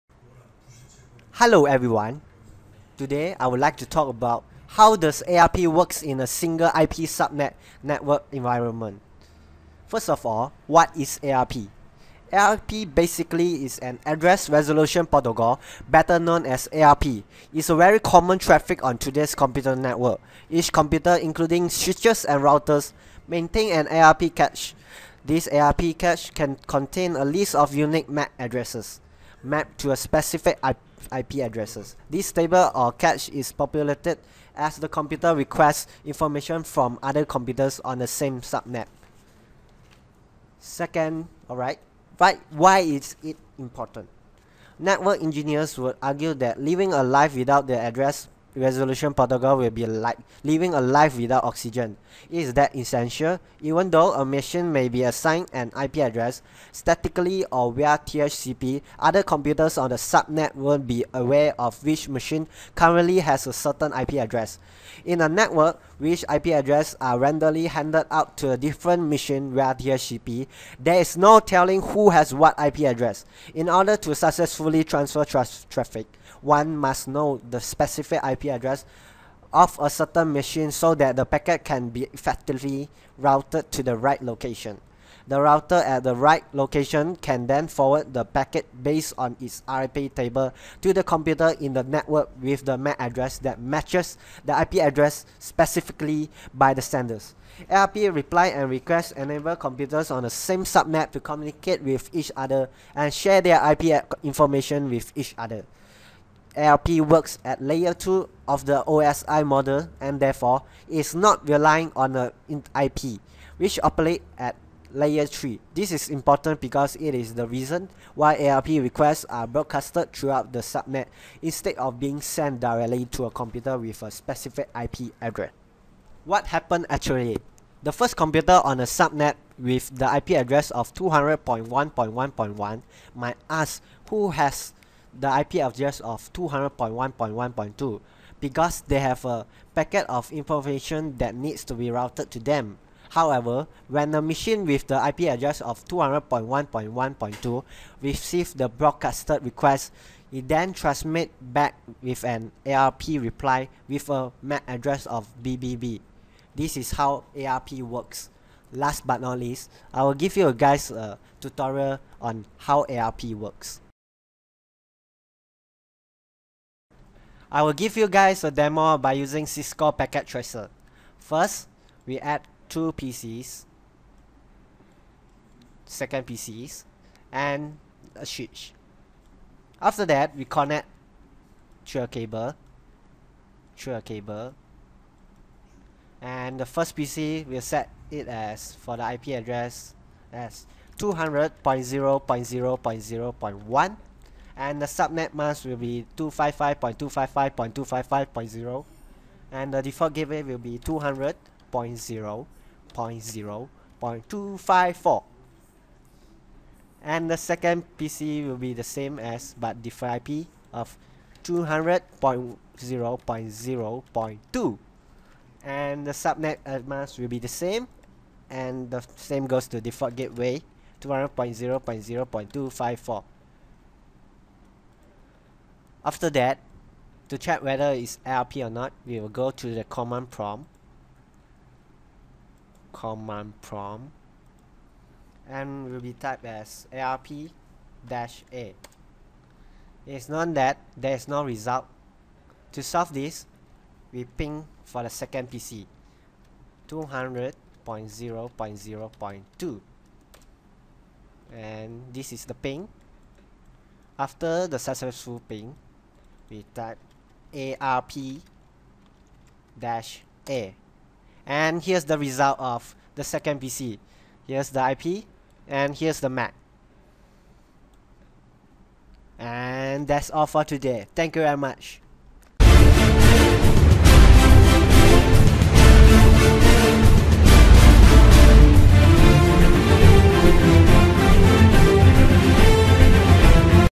ARP in 1 subnet environment (example 2)
What is ARP
Hello Everyone. First of all, what is ARP?? ARP basically is Address Resolution Protocol, better as known as ARP, is very common traffic on today's computer networks. Each computer (including switches and routers) maintains an ARP cache. This ARP cache contains a list of unique MAC addresses (Media Access Code) mapped to specific IP addresses. This table or cache is populated as the computer requests information from other computers on the same subnet.

Second, Why is it Important?? Network engineers would argue that living a life without the Address Resolution Protocol would be like living a life without oxygen... It's that essential. Even though a machine may be assigned an IP address statically or via DHCP, other computers on the subnet won't be aware of which machine currently has a certain IP address. In a network which IP addresses are randomly handed out to different machines via DHCP, there is no telling who has what IP address. In order to successfully transfer traffic, one must know the specific IP address of a certain machine so that the packets can be effectively routed to the right location. The router at the right location can then forward the packet based on its ARP table to the computer in the network with the MAC address that matches the IP address specified by the sender. ARP replies and requests enable computers on the same subnet to communicate with each other and share their IP information with each other. ARP works at Layer 2 of the OSI model, and therefore is not reliant on the Internet Protocol (IP), which operates at Layer 3. This is important, because it is the reason why ARP Requests are broadcasted throughout the subnet instead of being sent directly to a computer with a specific IP address.

What Happened Actually?
What happened actually?? The first computer on the subnet with the IP address of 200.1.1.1 might ask "who has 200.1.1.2 tell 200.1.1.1", because they have a packet of information that needs to be routed to them. However, when the machine with the IP address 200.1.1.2 receives the broadcasted request, he transmits back an ARP Reply with a mac address of "BBB". This is how ARP works. Last but not least, I will give you guys a tutorial on how ARP works.

CISCO Packet Tracer Demo
I will give you guys a demo using CISCO Packet Tracer. First, we add two PCs and a switch. After that, we connect through a cable. The first pc IP address will be set as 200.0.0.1.The subnet mask would be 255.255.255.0 and the default gateway would be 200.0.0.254.The second pc would be the same but in different IP with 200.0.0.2.The subnet mask will be the same and same goes to default gateway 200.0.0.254. After that, to check whether it is ARP or not, we will go to the command prompt. Then, we will type as "arp --a". It is known that no result. To solve this, we ping for the second pc by key in "ping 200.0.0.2". Then, this is the ping that we request for. After the successful ping, we type "arp --a". This is the result of second pc. Here is the IP address and here is the MAC address. That is all for today. Thank you very much.